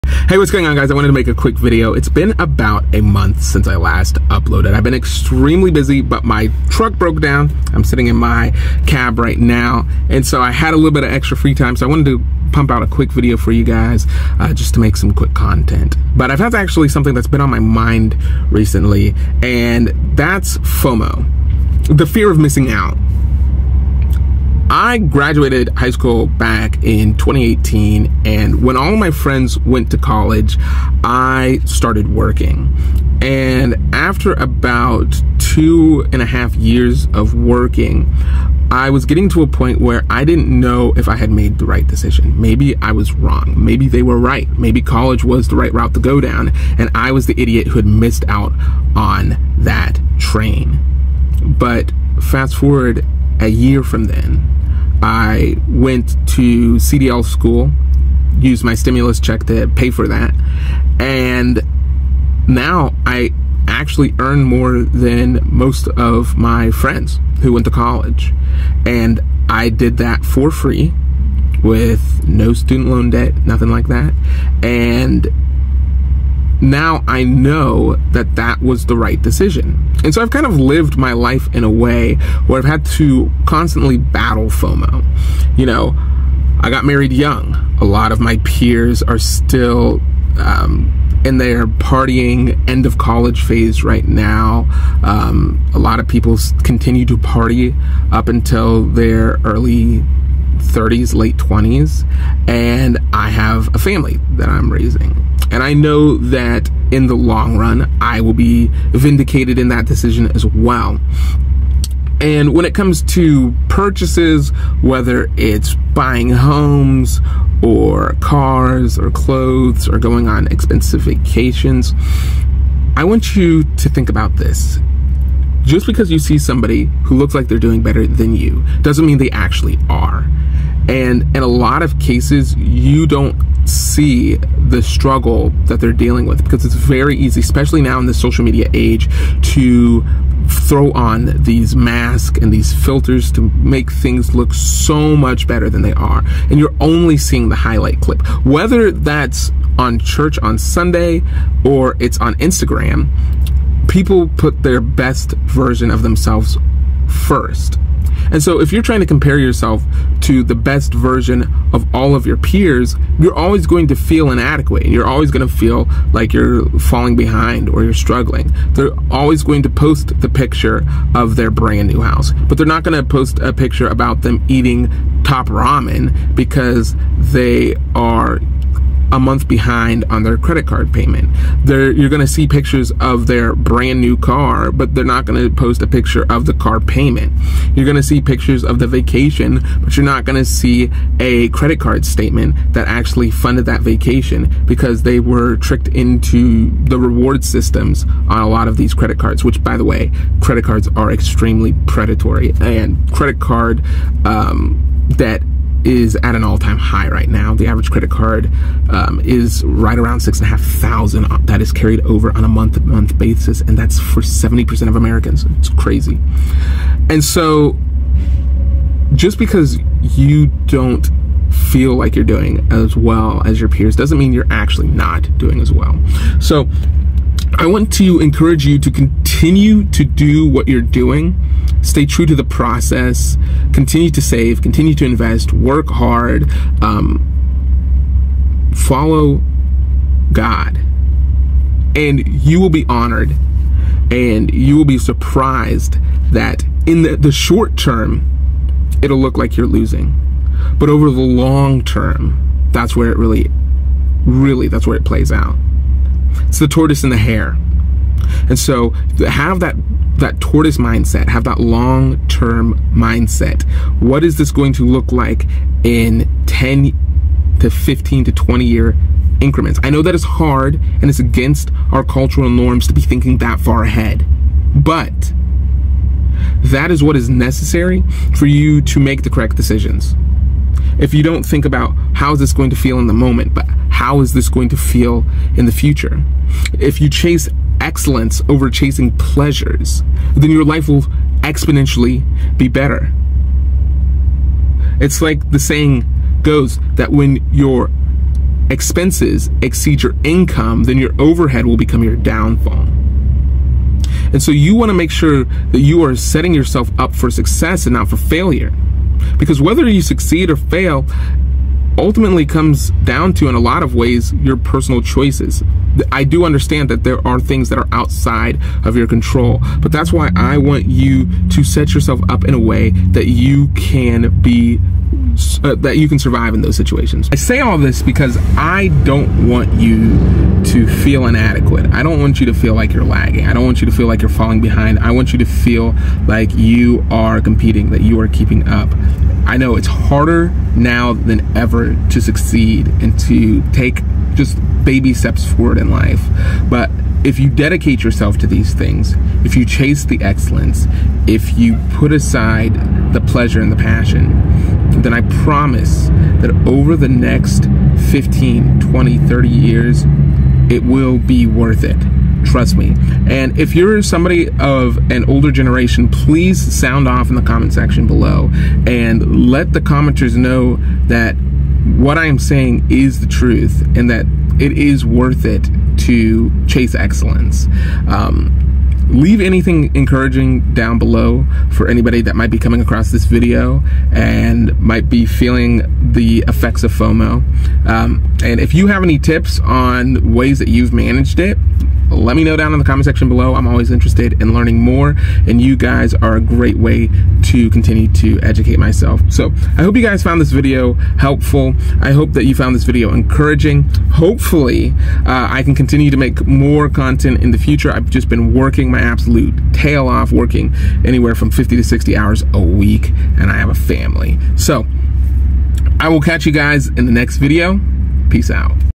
F.O.M.O [This can ruin your life]
If you're currently struggling with or have struggled with Fomo don't worry you're not alone. This very human feeling of the need to be an active participant in the community and to look good to others has been plaguing us since the dawn of time but through dedication and patience you will be rewarded for taking the measured approach to  life the balanced line and you will be more successful than you could possibly imagine in the future.